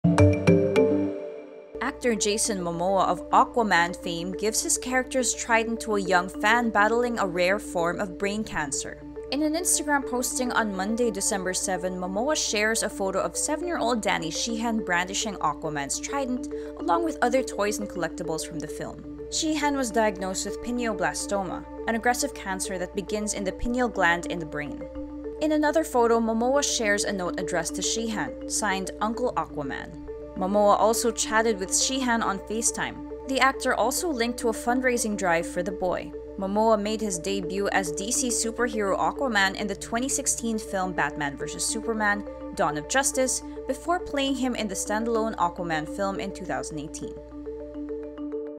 LOOK: Aquaman Jason Momoa gives trident to boy with brain cancer
More videos on Rappler Actor Jason Momoa of ‘Aquaman’ fame gives his character’s trident to a young fan battling a rare form of brain cancer.